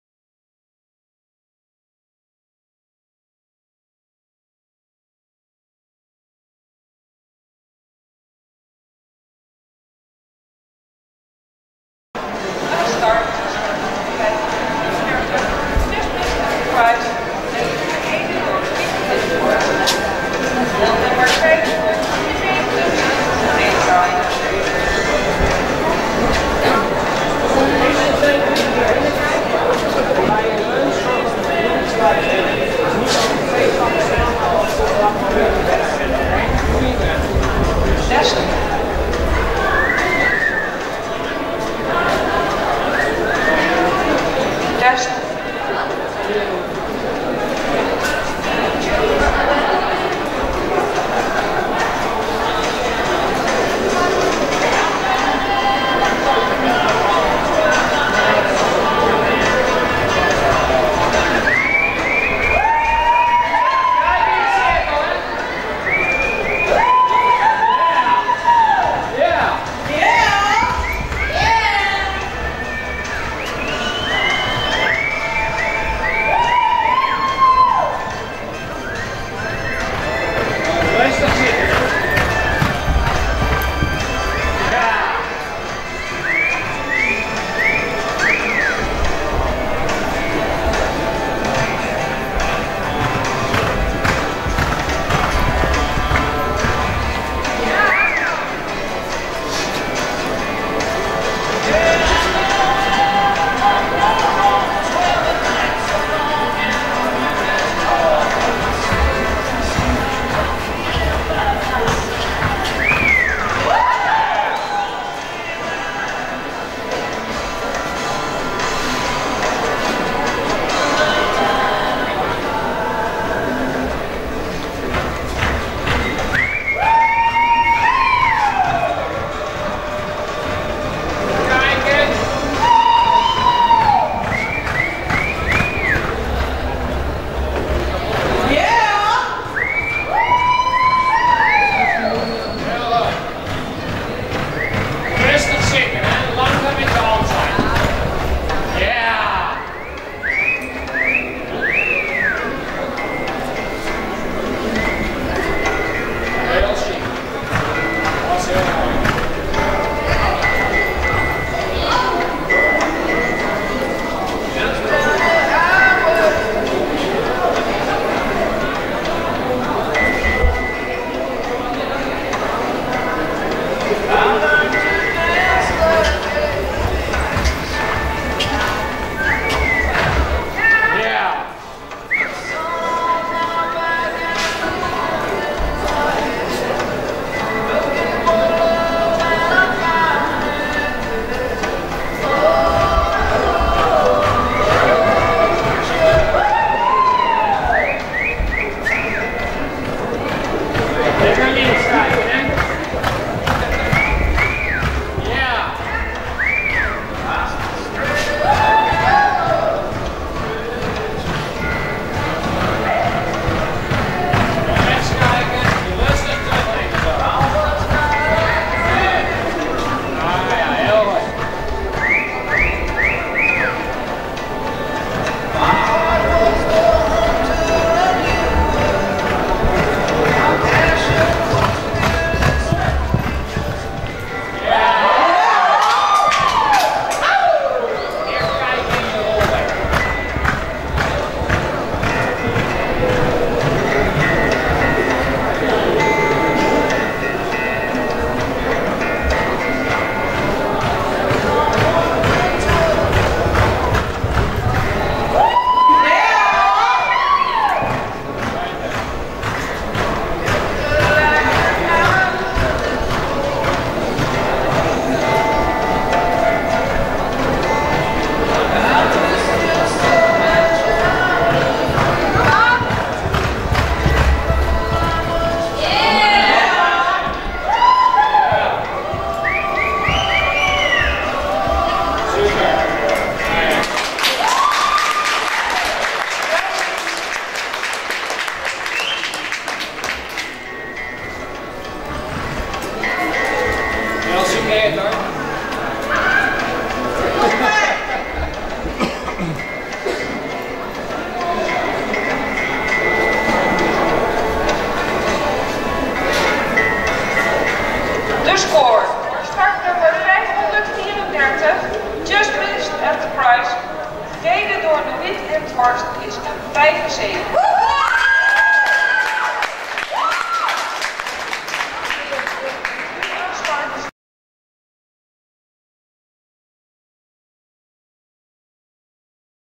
Just Miss Enterprise - Non Pro Champions - Score 75!
Nuith Linthorst & Just Miss Enterprise
2007 QH Mare by Surprise Enterprise

DRHA Summer Slide
Non Pro Champions with a score of 75!